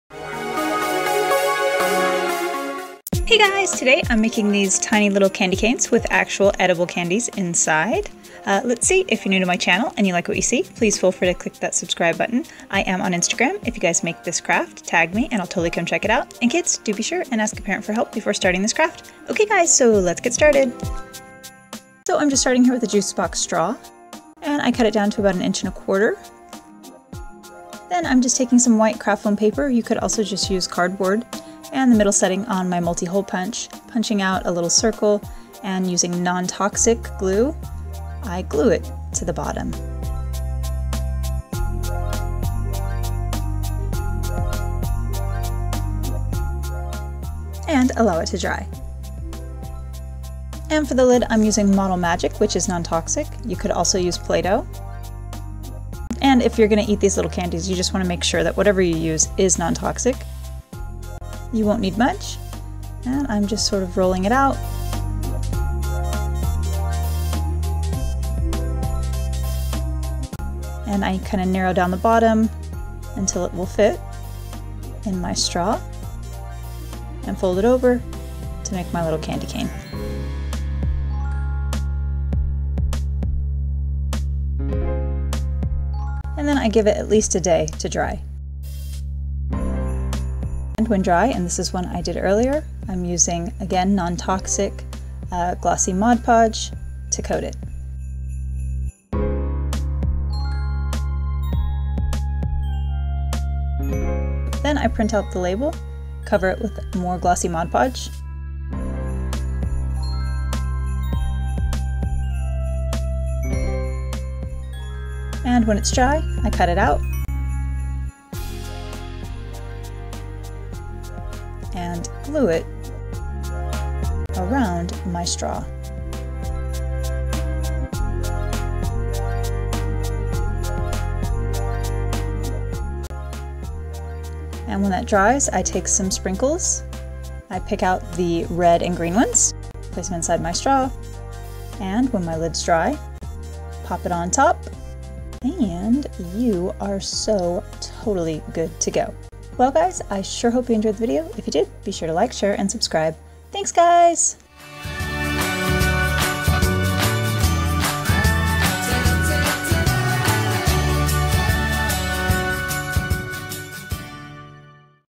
DIY Miniature M & M Candy Canes (w Real Candies Inside)! - Christmas Holiday Craft
Hey guys!  Today, I'm making DIY miniature Christmas holiday dollhouse candy canes with candy inside.

Supplies: straws, craft foam, non-toxic glue, non-toxic Model Magic, real candies (sprinkles), glossy Mod Podge, & a printable.